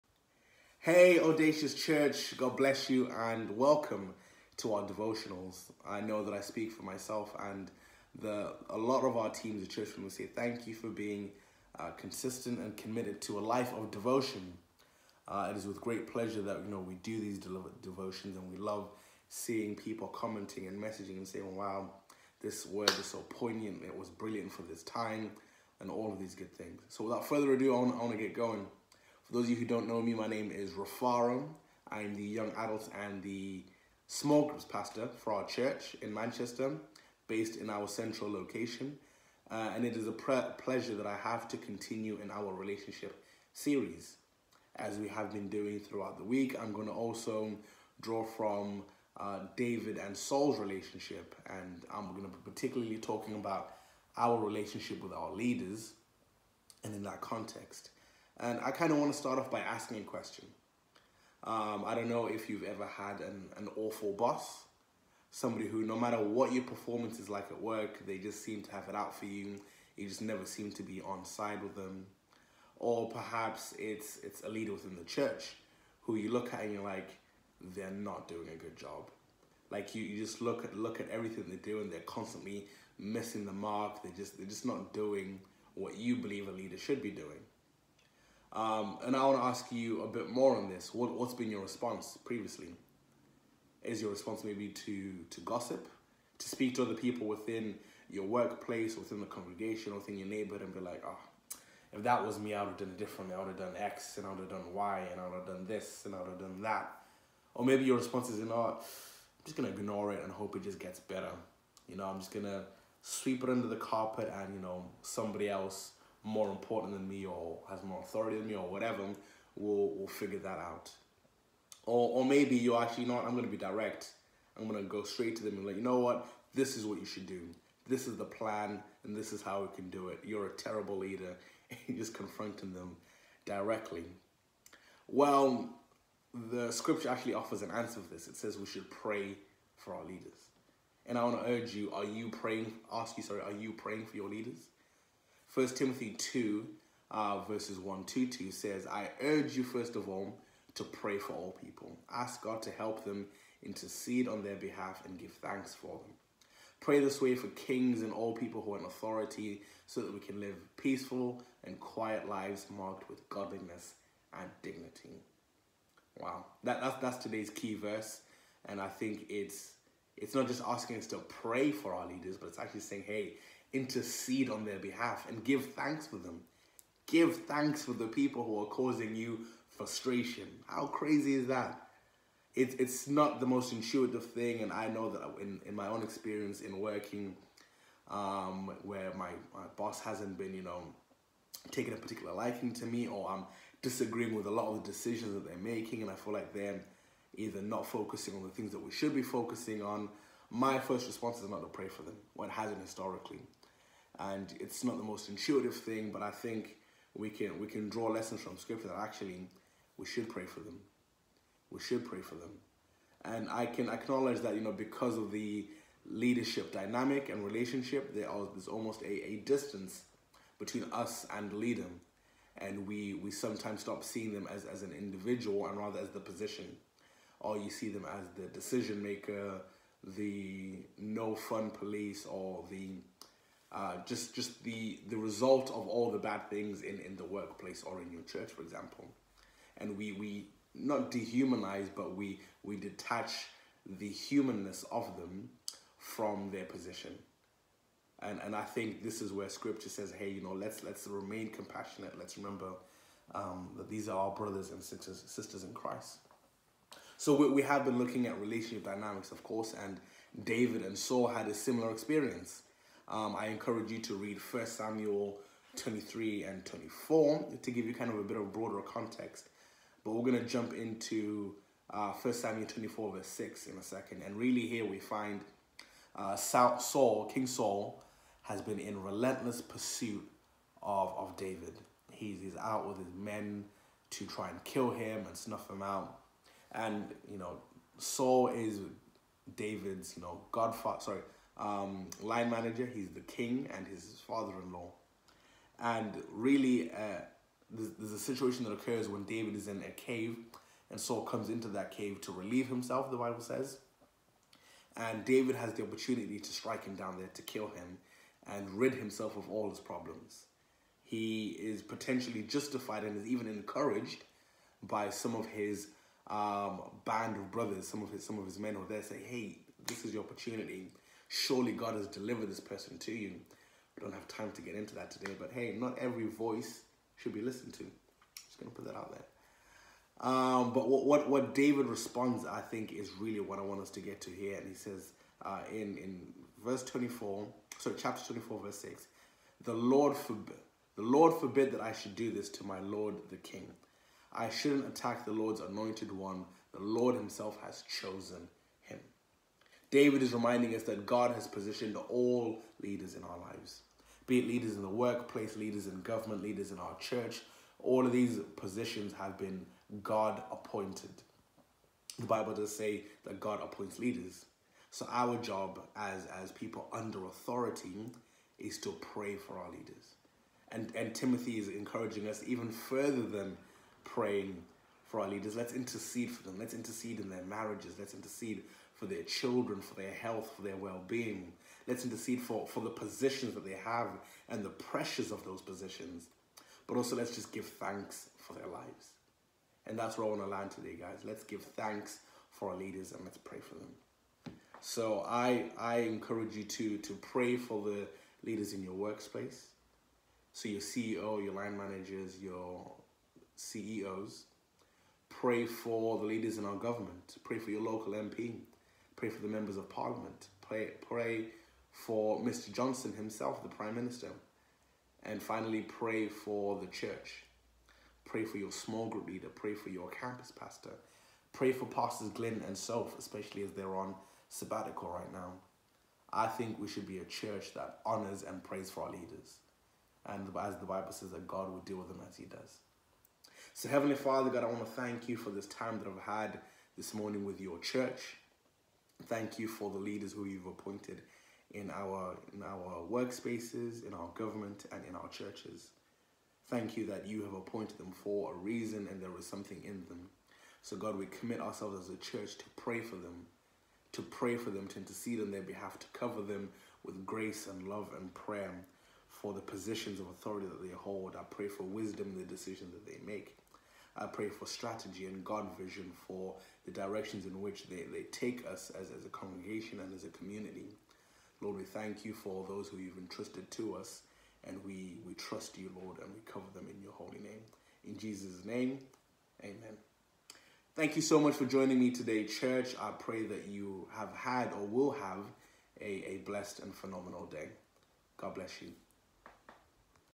!Audacious Daily Devotionals - Friday 24th June 2022
Today, Rufaro Jerahuni our Central Manchester Campus Young Adults Pastor continues our devotional series on relationships.